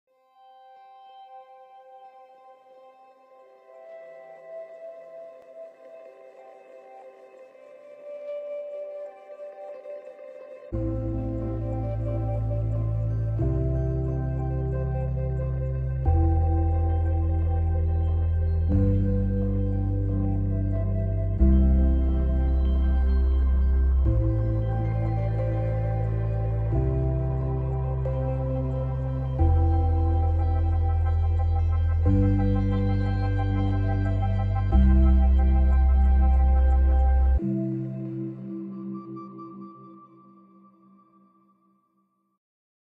Flying above the Greater Himalayan Ranges
The high altitude mountains appear so majestic and pure spreading their magical spell in all directions . Feeling totally connected with the grandeur design of nature .